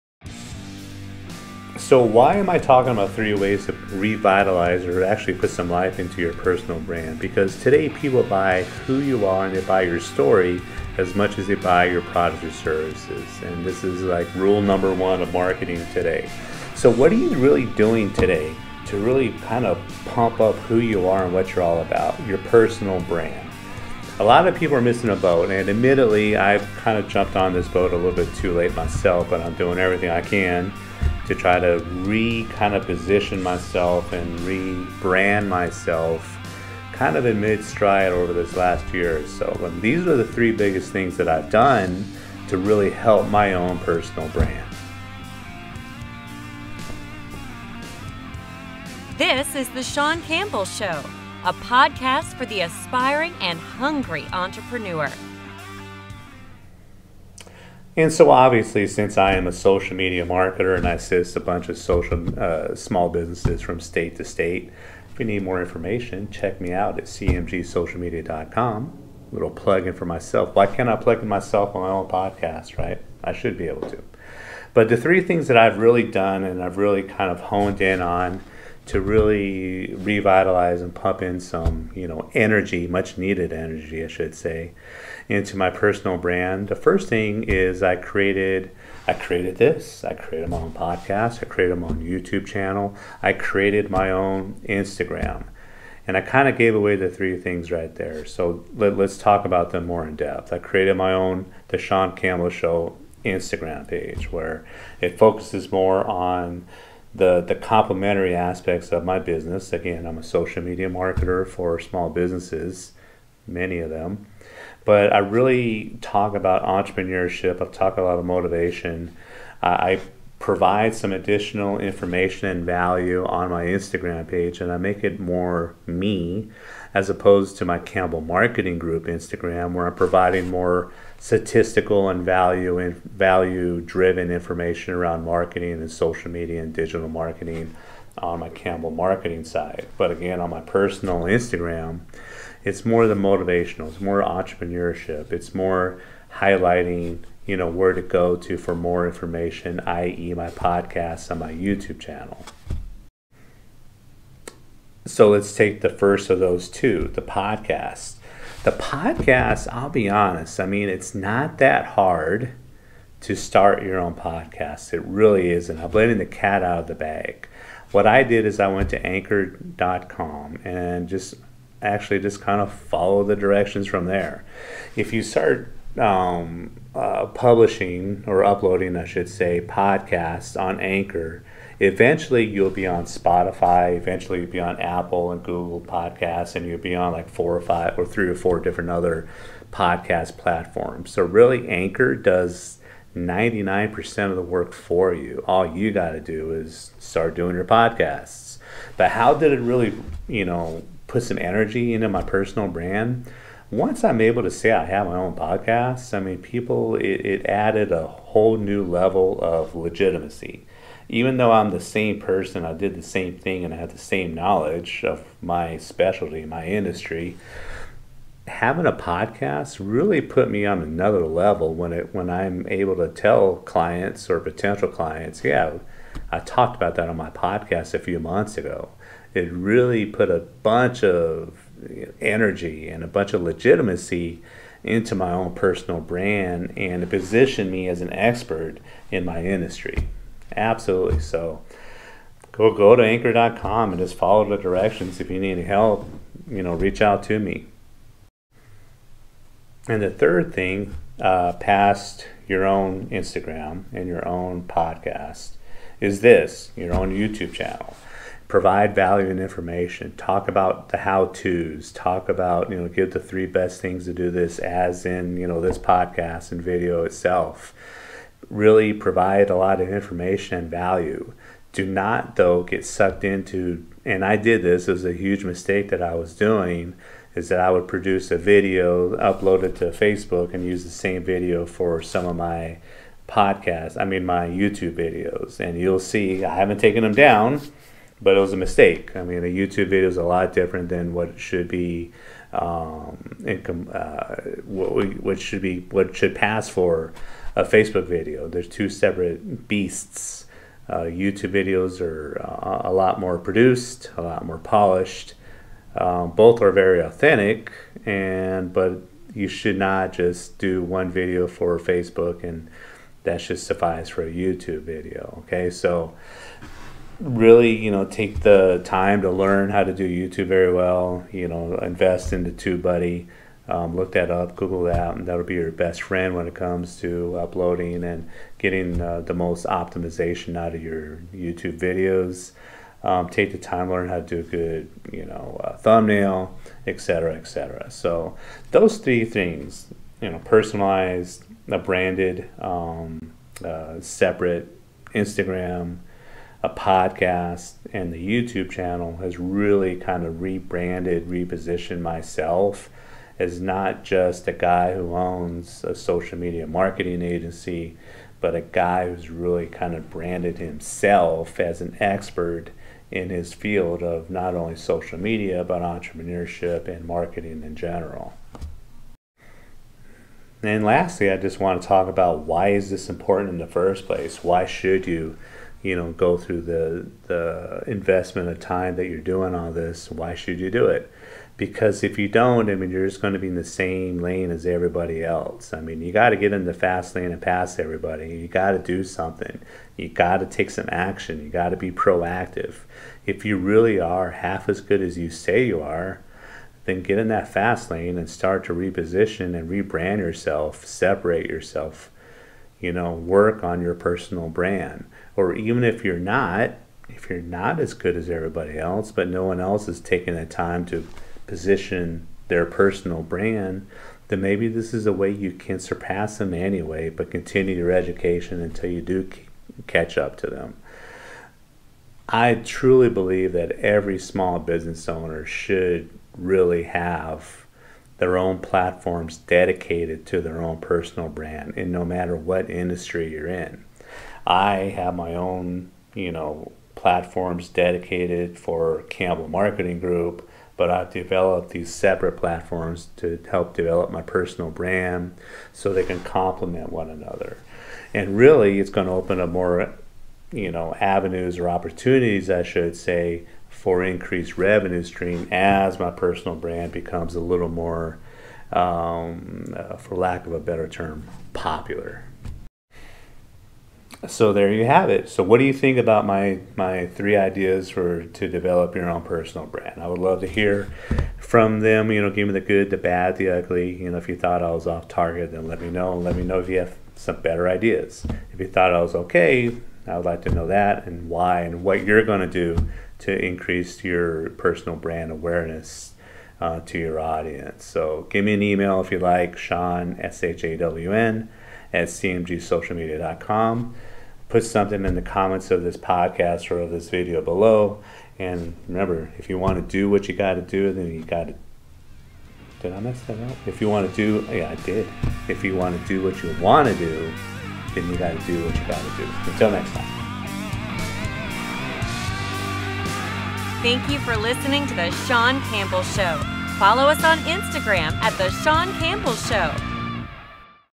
3 Ways an Entrepreneur Can Energize Their Personal Brand
As an entrepreneur these days, you need to be paying attention to your personal brand as much as the business' brand. Here are three ways you can energize your brand as an entrepreneur and move yourself into the upper atmosphere of entrepreneurship!

And, of course, I cover three platforms and ideas on how to implement them for your personal brand - and not just for your small business. Remember, people buy who and what you're all about before they buy your products or services - they buy your "why". So, of course, energizing your personal brand is key in our era!